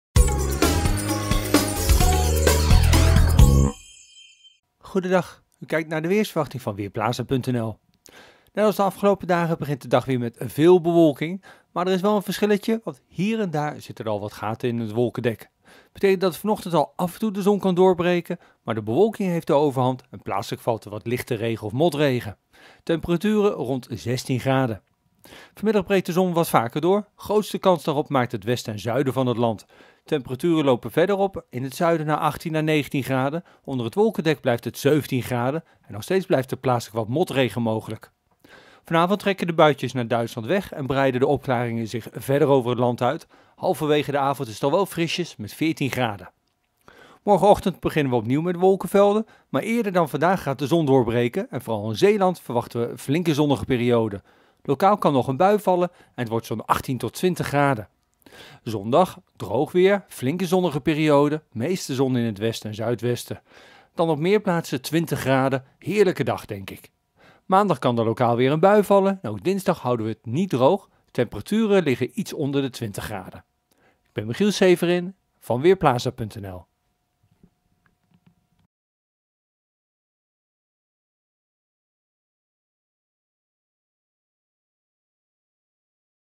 Weerbericht vrijdagochtend: Vanmiddag vaker zon
Wolkenvelden hebben de overhand en soms valt er wat motregen. Het droge weer overheerst en geleidelijk breekt vooral in het westen en zuiden de zon vaker door. Het wordt 16 tot 18 graden. De noordwestenwind is zwak tot matig, 2 tot 4 Bft, langs de noordkust soms vrij krachtig, 5 Bft. In het weekend schijnt de zon overal vaker en blijft het zo goed als droog. Vooral zondag wordt een dag met geregeld zon en weinig wind. De temperatuur ligt dan tussen 18 en 21 graden.